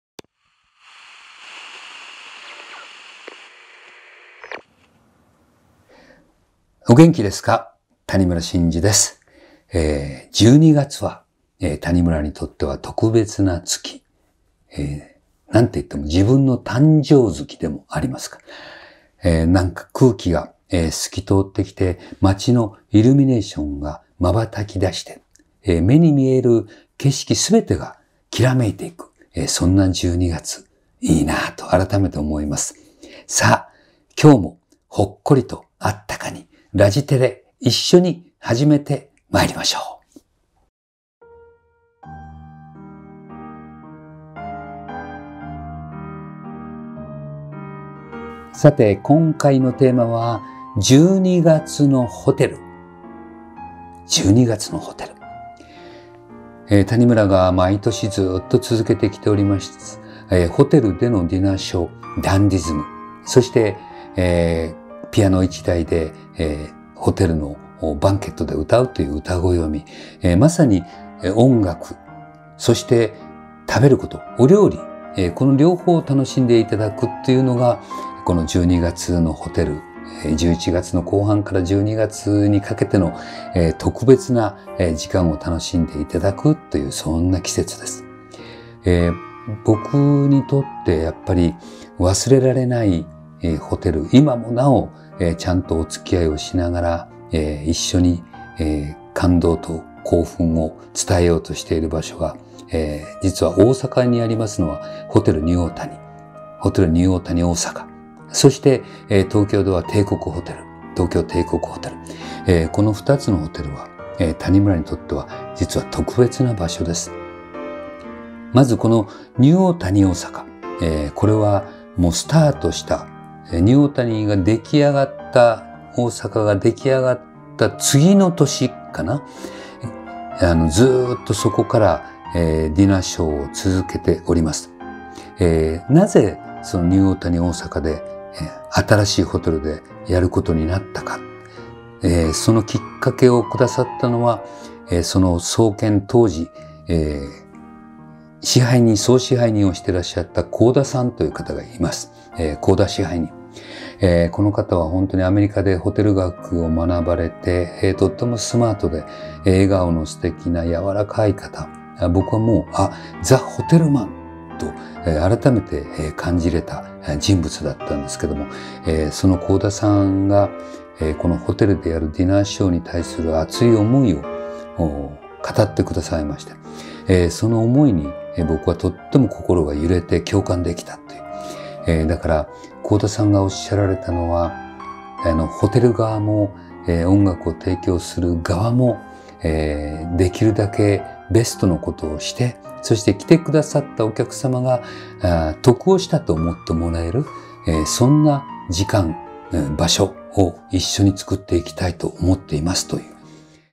谷村新司の【ラジテレ】#41「12月のホテル」 ダイジェスト版
#41「12月のホテル」

リアルタイムの谷村新司が見れる
タニテレ人気コンテンツのラジテレ第４１話！
今回は「１２月のホテル」がテーマ！まもなく行われる「DANDYISM2021」そこで帝国ホテルでの思い出やシェフとの
対談の裏話などこのラジテレだけに語ってくれました！

皆さんもディナーショーに参加してみてはいかがですか？